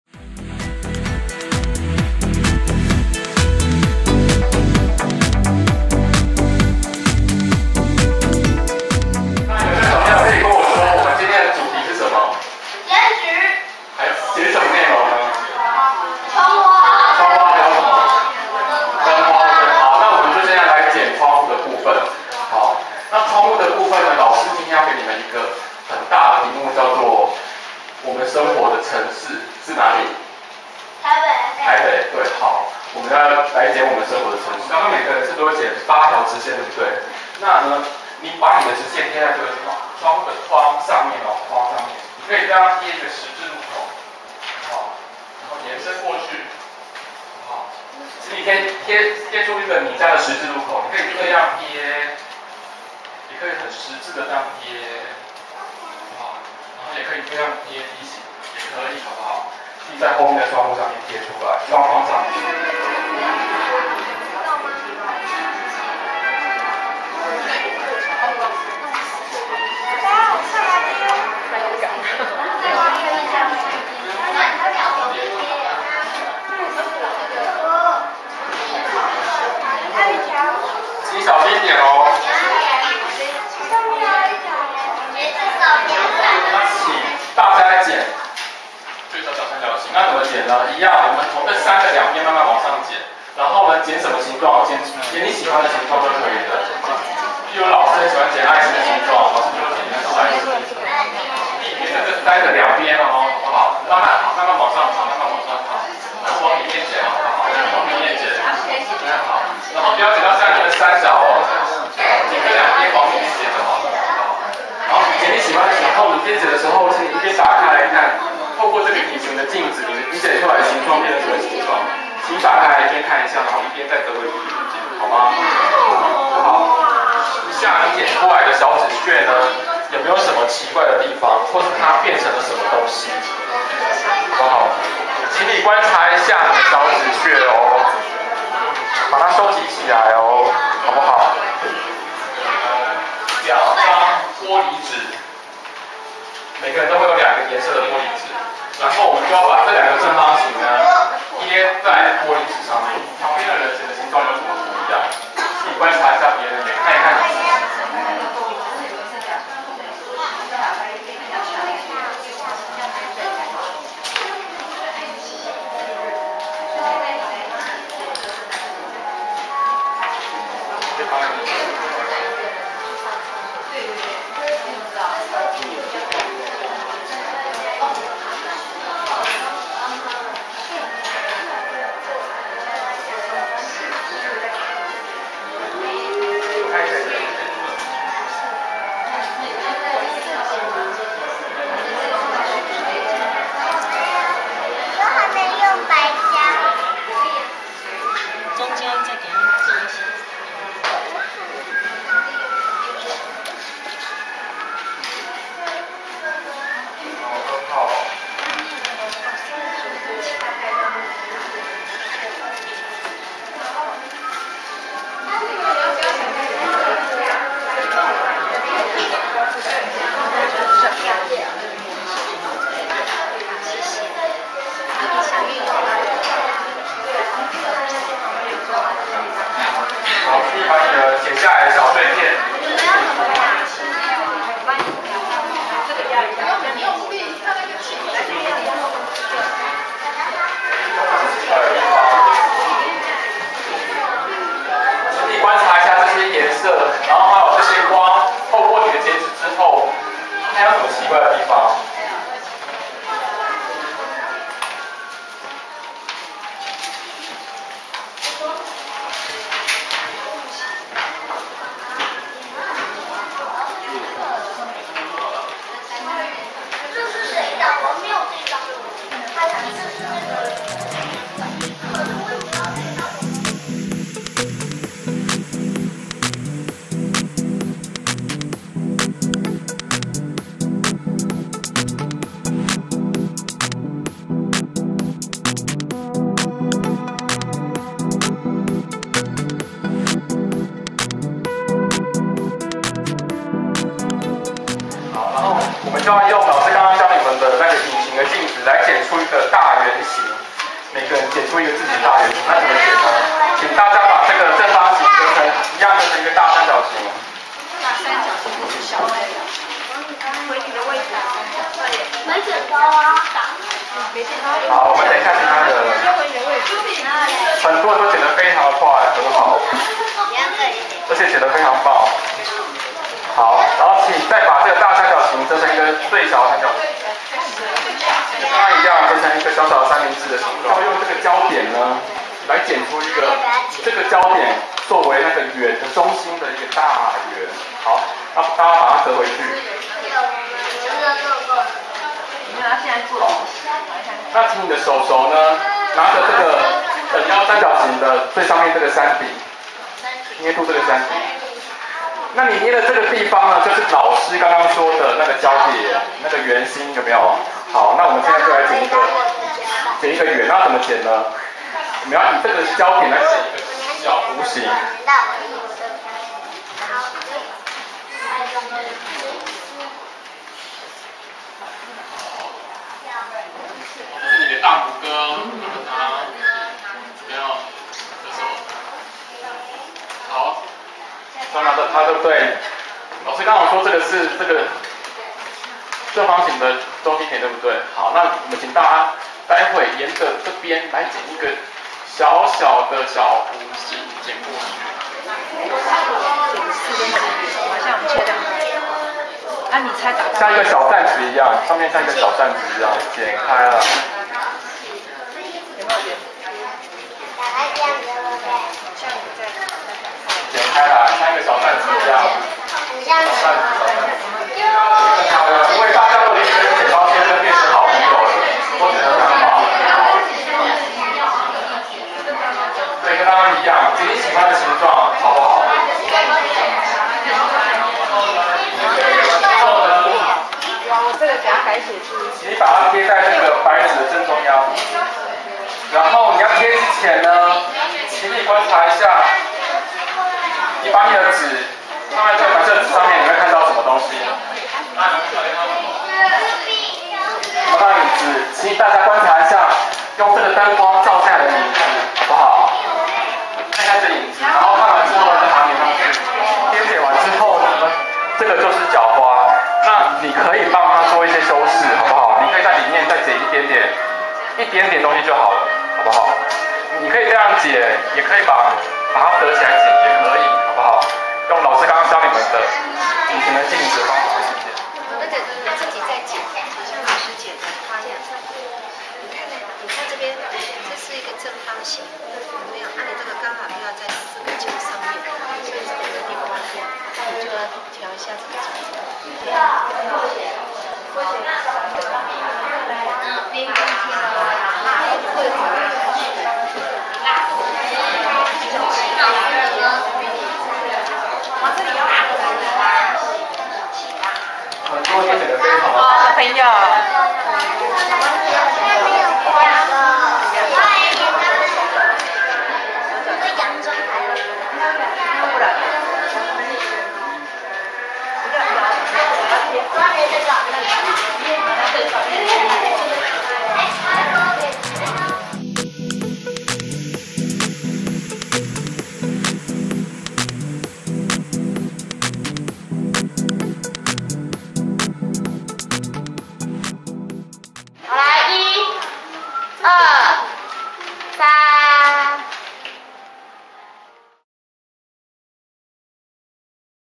台北當代藝術館_週末工作坊_窗花燈花光作坊
活動時間：2008年8月10日下午2:00-5:00
講師：吳耿禎
剪城市--窗花燈花光作坊
一半紙加一半光線
張開來就是一座城市
開了窗嗎  開了門嗎  開了燈嗎  點了月亮嗎
關了窗嗎  關了門嗎  關了燈嗎  熄了月亮
從紙縫間溜出來的光
照不亮整座夜

課程簡介：利用存在於建築物室內的兩種光---白天自然光(窗戶)和夜晚燈光(燈泡、 燈座 、日光燈)來創作，將剪紙附緣於這兩種光源上，一為窗花一為燈花。